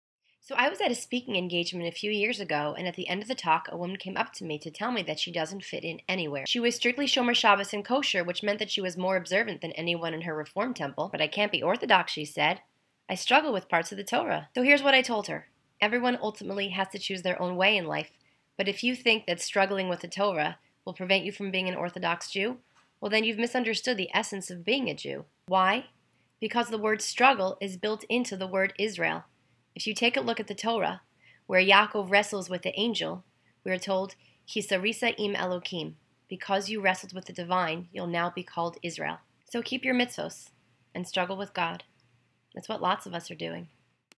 Can Orthodox Jews Struggle With Torah?
JewintheCity.com A woman once told me that she's too religious for her Reform temple but because she struggles with the Torah, she can't be Orthodox...